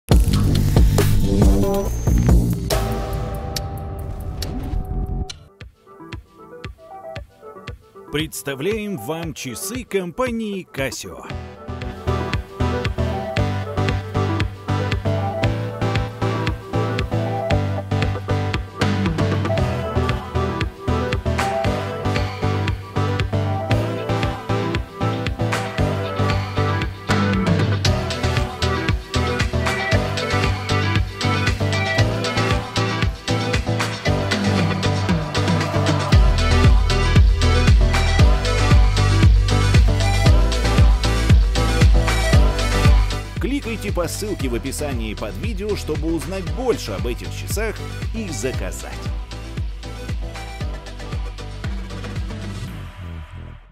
Обзор часов Casio Edifice EFS-S560D-1AVUEF с хронографом. Японские наручные часы. Alltime
⌚В этих часах надежность гармонично сочетается с функциональностью и быстродействием. Edifice. Новинка в коллекции часов Edifice. Ультратонкие. Питание от солнечной энергии, индикатор уровня зарядки батареи. Необритовое светонакопительное покрытие обеспечивает длительное послесвечение в темноте даже после кратковременного нахождения на свету. Часы-хронограф с секундомером с временем измерения 30мин. Ретроградный указатель минутной стрелки хронографа. Окошко даты. Заводная головка с защитой. Раскладывающаяся застежка, расстегиваемая одним касанием. Застежка с системой блокировки.

AllTime. ru

20 лет безупречной работы! Свыше 300 000
выполненных заказов!